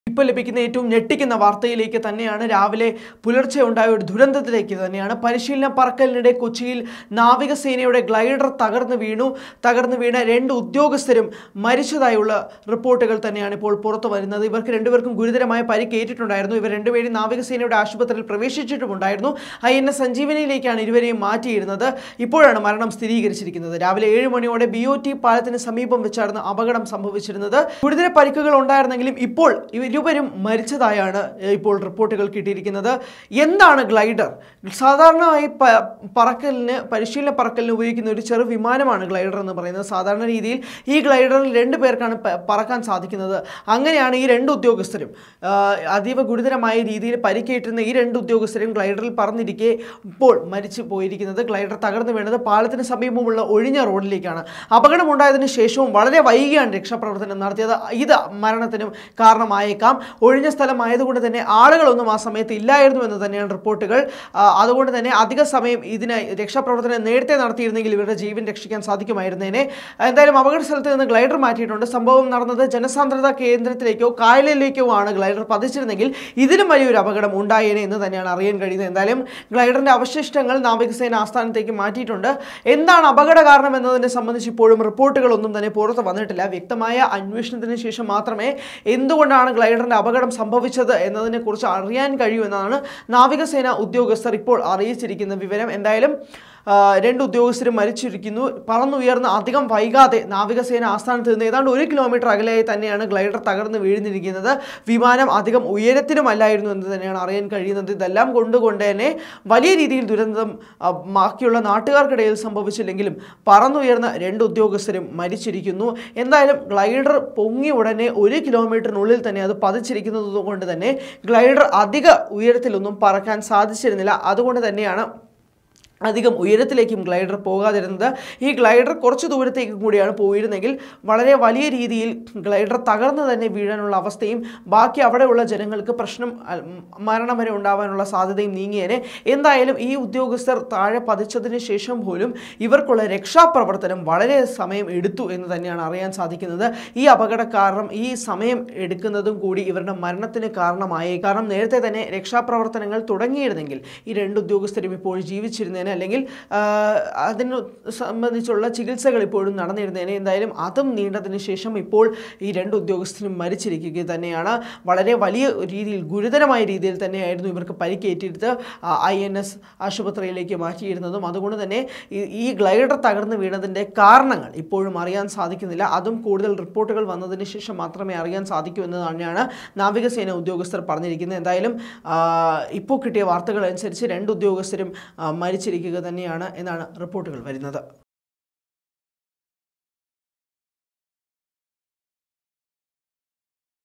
കൊച്ചിയിൽ ഗ്ലൈഡർ തകർന്ന് വീണ് രണ്ട് ഉദ്യോഗസ്ഥർ മരിച്ചു l navy glider crash
രാവിലത്തെ പരിശീലന പറക്കലിനിടെ കൊച്ചിയിൽ നാവികസേനയുടെ ഗ്ലൈഡർ തകർന്ന് വീണു. രണ്ട് ഉദ്യോഗസ്ഥർക്ക് ഗുരുതരമായ രീതിയിൽ തന്നെ പരുക്ക് ഇട്ടിട്ടുണ്ട്. ഇവർ രണ്ടുപേരെയും നാവികസേനയുടെ ആശുപത്രിയിൽ പ്രവേശിപ്പിച്ചിട്ടുണ്ട്. ഐഎൻഎസ് സഞ്ജീവനിയിലേക്കാണ് ഇരുവരെയും മാറ്റിയത്.  രാവിലെ 7 മണിയോടെയാണ് ബി.ഓ.ടി. പാലത്തിന് സമീപത്താണ് അപകടം സംഭവിച്ചത്.